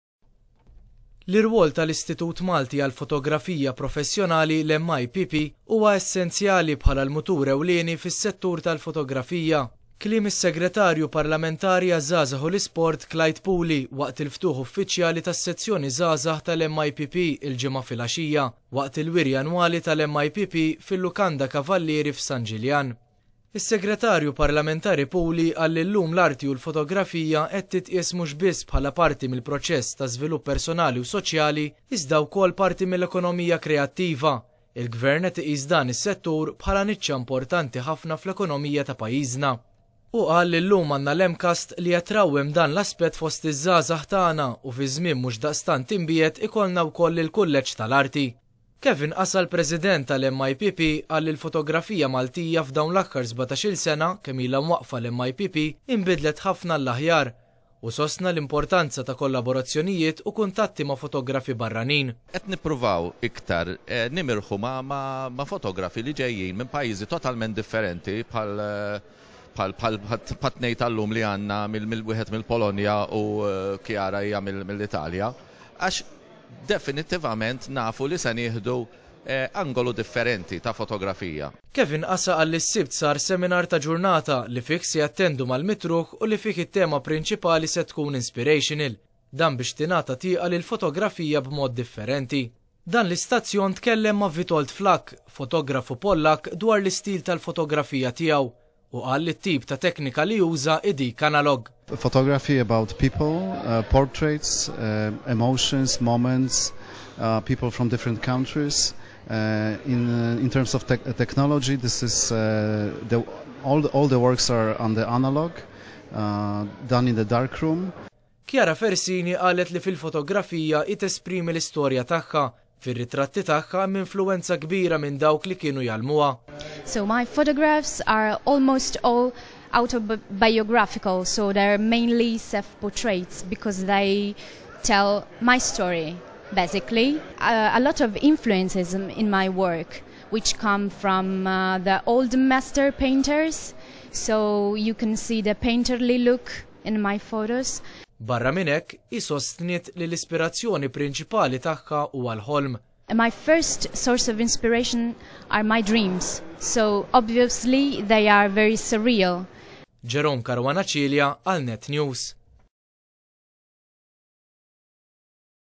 MIPP ANNUAL EXHIBITION / Wirja annwali mill MIPP
MIPP's Annual Exhibition, held at Hotel Cavalieri, St. Julians.